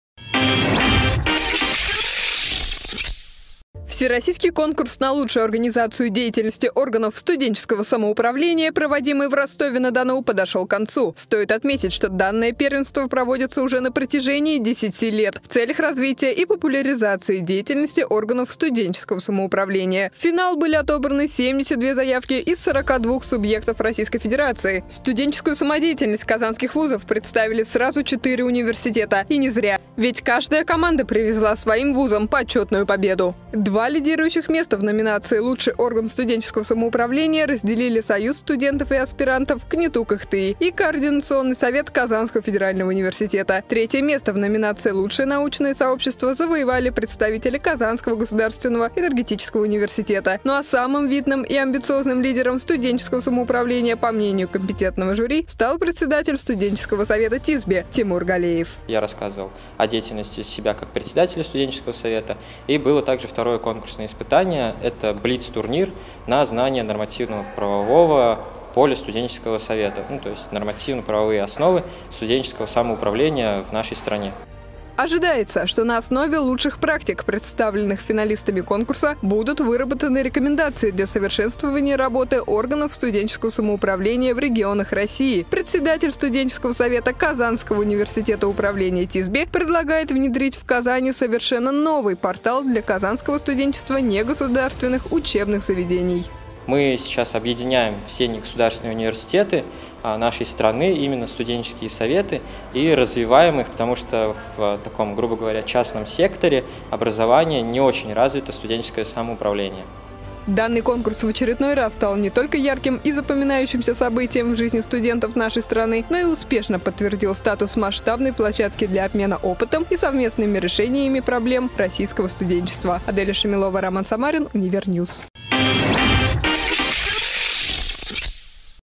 Громкие результаты студенческого самоуправления
Негосударственные учебные заведения Казани должны объединить воедино весь потенциал и создать абсолютно новое студенческое пространство – заявляет лидер студенческого совета ТИСБИ. Эта идея была заслужено оценена на Всероссийском конкурсе студенческого самоуправления в Ростове-На-Дону. Чем запомнилось прошедшее мероприятие в этом году, расскажет Аделя Шемелова